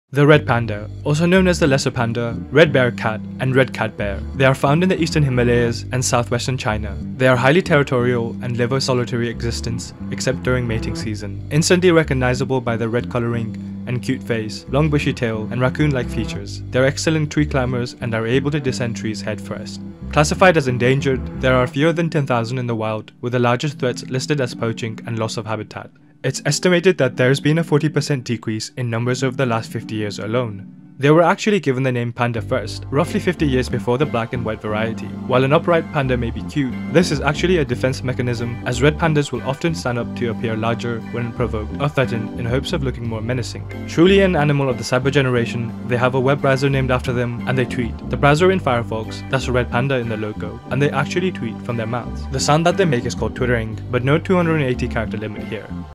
Facts about the Red Panda 🐼
The Red Panda. The red panda, also known as the lesser panda, is a small mammal native to the eastern Himalayas and southwestern China. It
has dense reddish-brown fur with a black belly and legs, white-lined ears, a mostly white muzzle and a ringed tail. Unfortunately, their current status is
endangered, with numbers decreasing year on year due to poaching and deforestation.